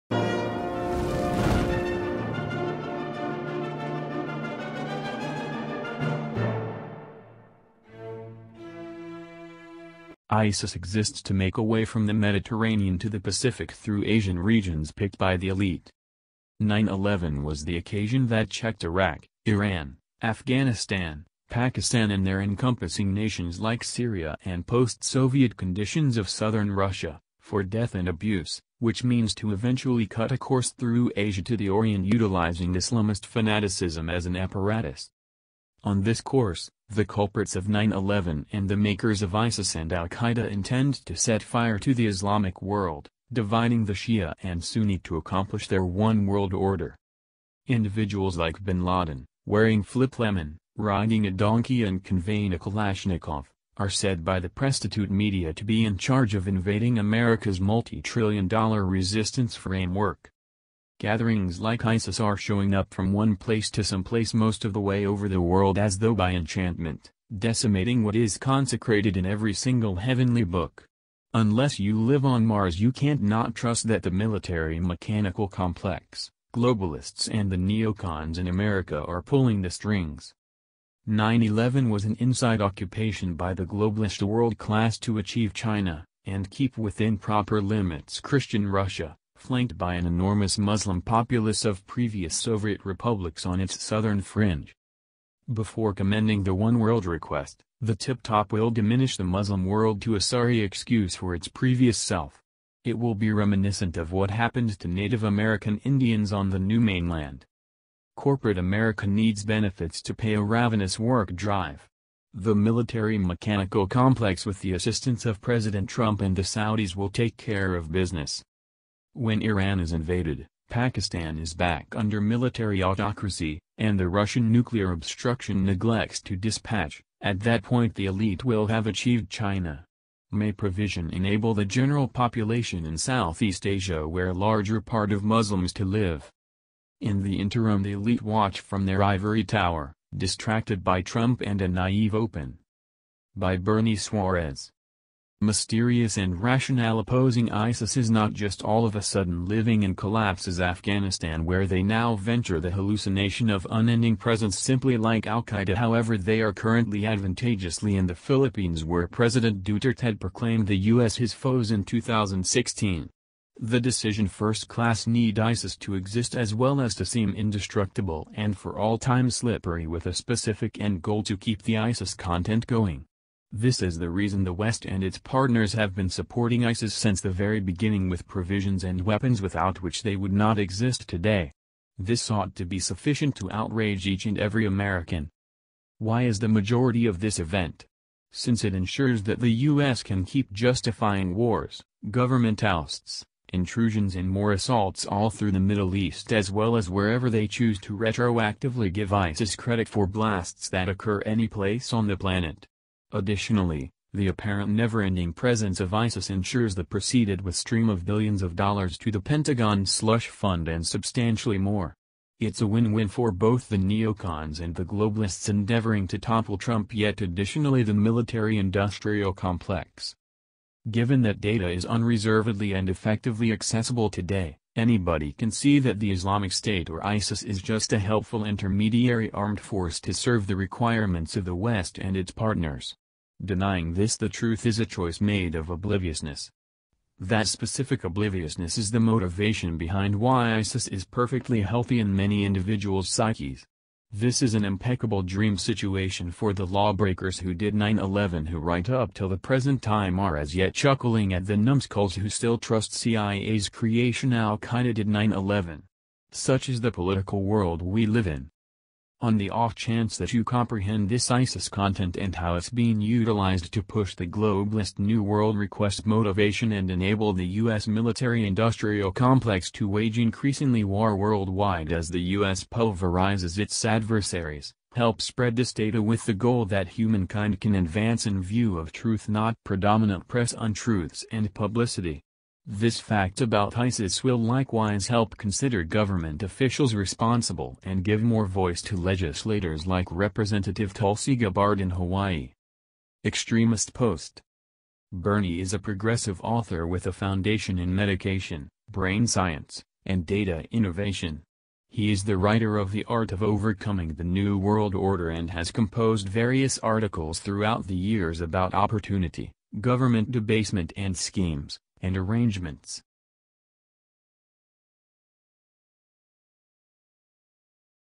ISIS Works For Western Elite To Organize Global Terror | Must-See !!
ISIS exists to make a way from the Mediterranean to the Pacific through Asian regions picked by the Elite.